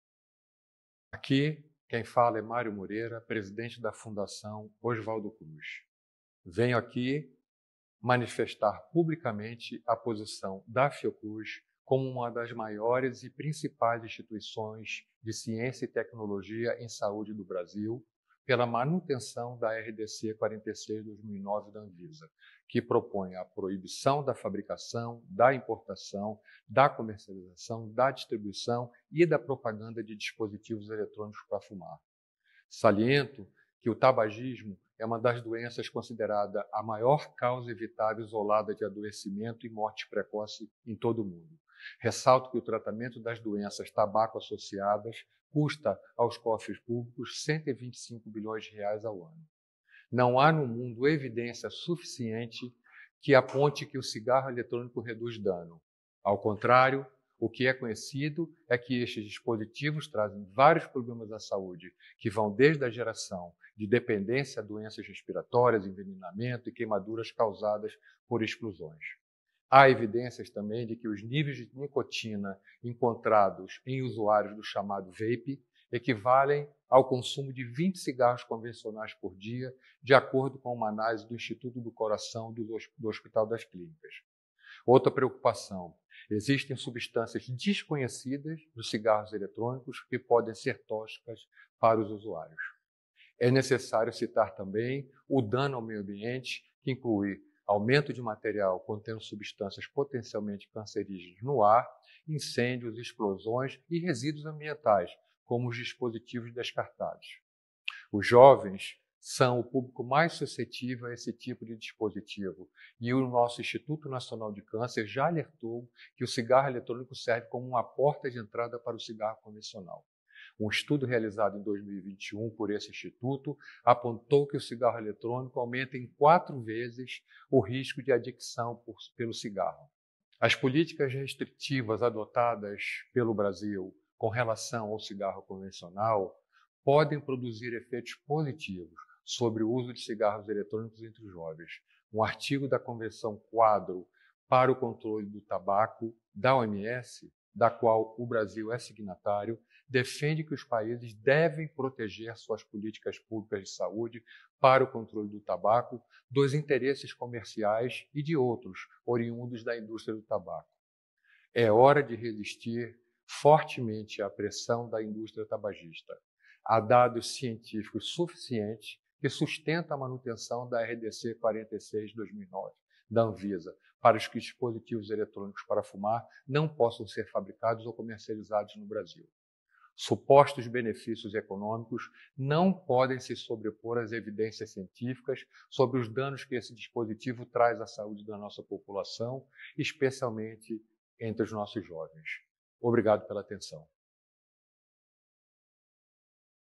CAT_ Fiocruz apoia RDC 46 da Anvisa: NÃO AO CIGARRO ELETRÔNICO NO BRASIL
Presidente da Fundação Osvaldo Cruz, Mário Moreira, expõe publicamente a posição da Instituição, que é a de apoio à manutenção da Resolução da Diretoria Colegiada de número 46, de 2009, a que proíbe importação, publicidade, distribuição e venda de todo e qualquer dispositivo eletrônico para fumar - DEF, em todo território nacional.